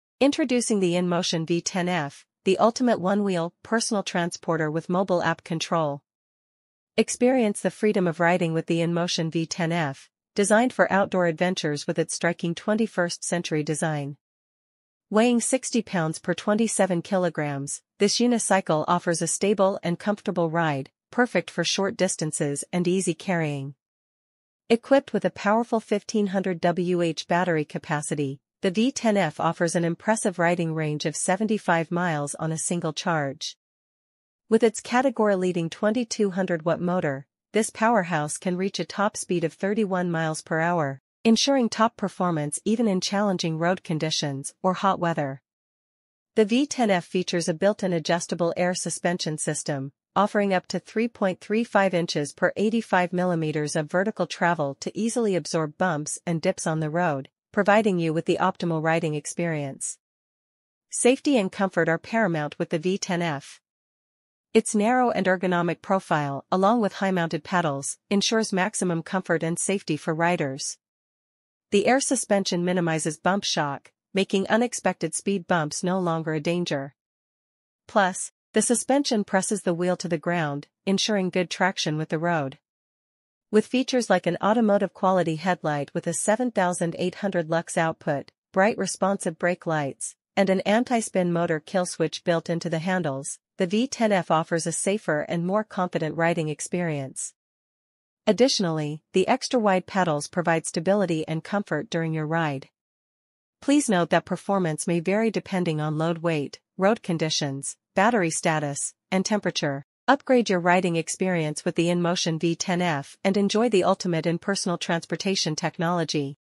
Master Urban Travel: INMOTION V10F, Your Personal Transport Companion!||Toys For Kids
Get ready to revolutionize your daily commute with the INMOTION V10F, the ultimate one-wheel personal transporter with mobile app control! Say goodbye to traditional commuting hassles and embrace seamless urban mobility like never before. Whether it's navigating busy city streets or exploring your neighborhood, the INMOTION V10F offers unparalleled convenience and style. Join the movement towards a smarter, more efficient way of transportation today!